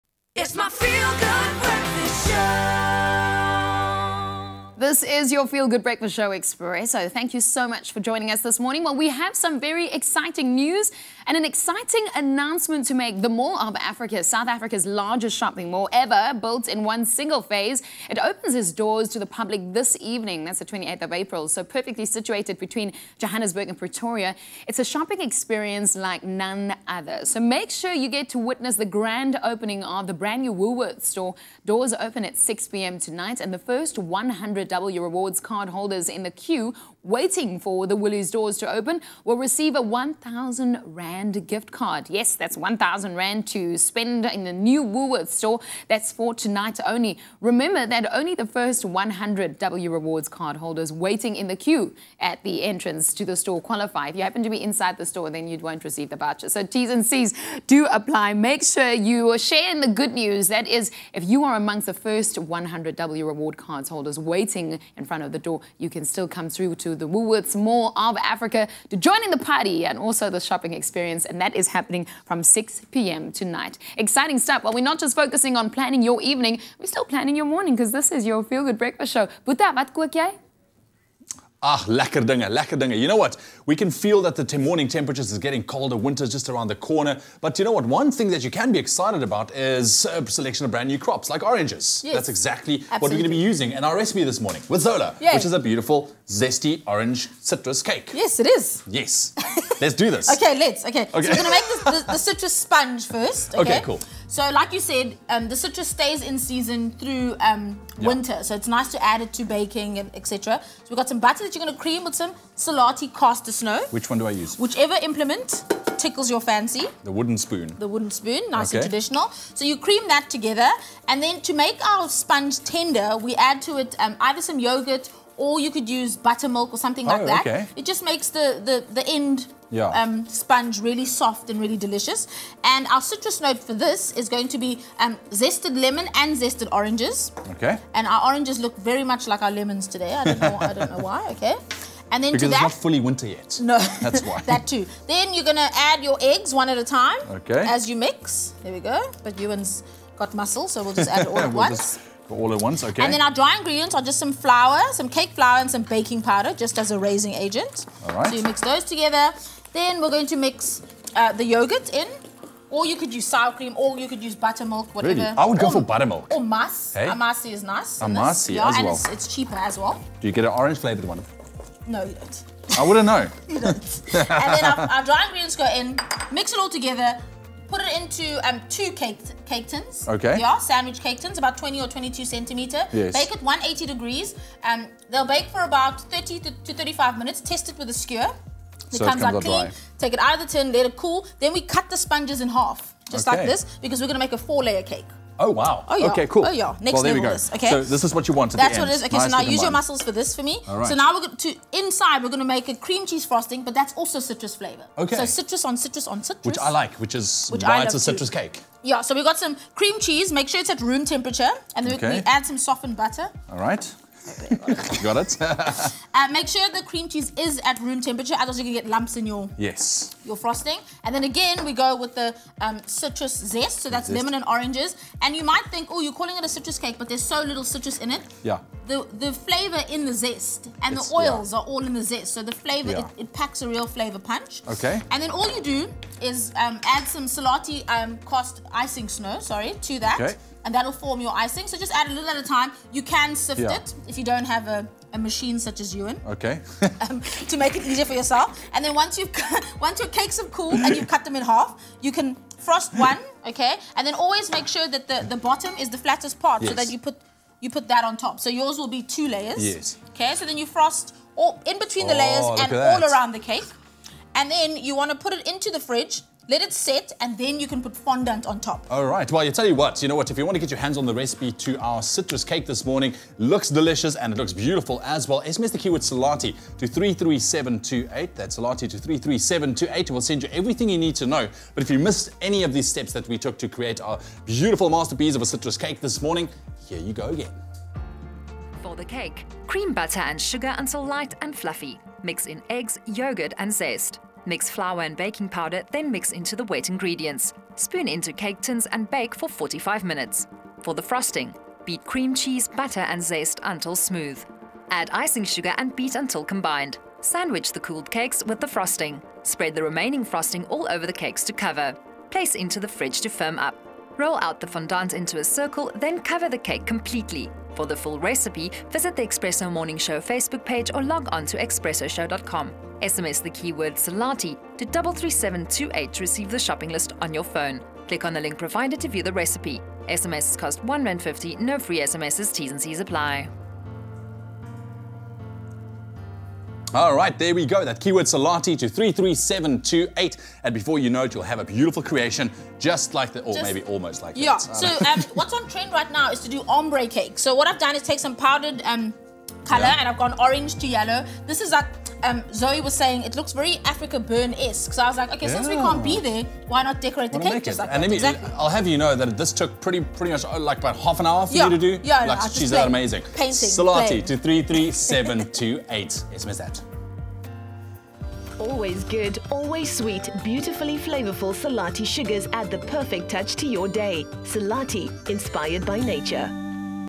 Citrus Cake by Zola Nene
With winter just around the corner, we’re waving goodbye to summer, but these frosty mornings bring the promise of new and exciting seasonal crops like oranges making baking all the more fun. One of our favourites? Zola’s Citrus Cake!